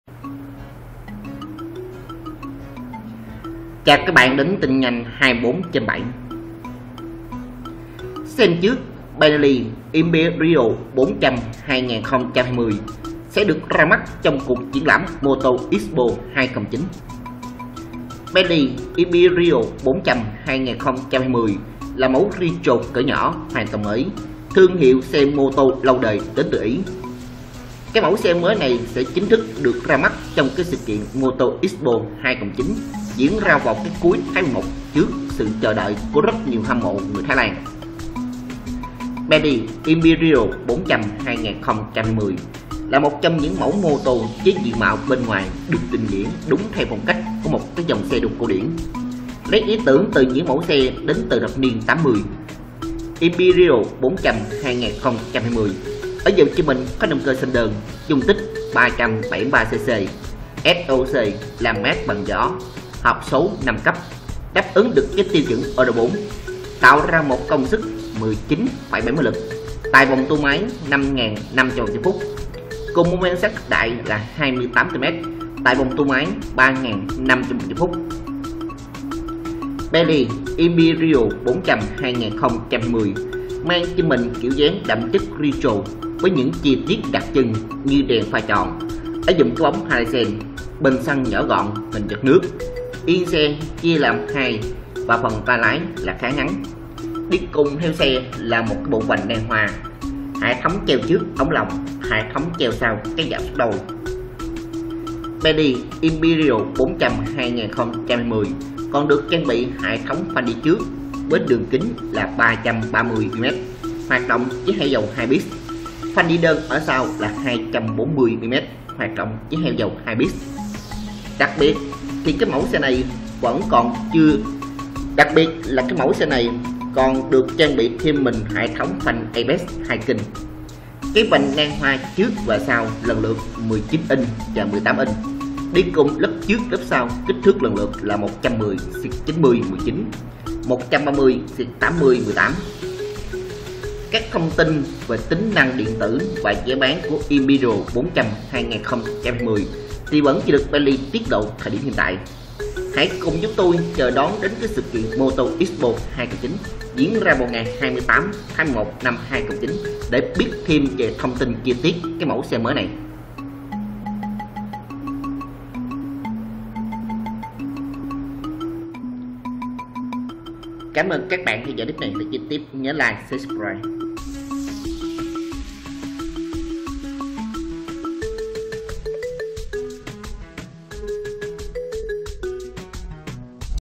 Tin nhanh 24/7 - Xem trước Benelli Imperiale 400 2020 sẽ ra mắt tại Motor Expo 2019.
Benelli Imperiale 400 2020 là mẫu xe Retro cỡ nhỏ hoàn toàn mới của thương hiệu xe mô tô lâu đời đến từ Ý, mẫu xe mới này sẽ chính thức được ra mắt tại sự kiện Motor Expo 2019 diễn ra vào cuối tháng 11 trước sự chờ đợi của người hâm mộ Thái Lan.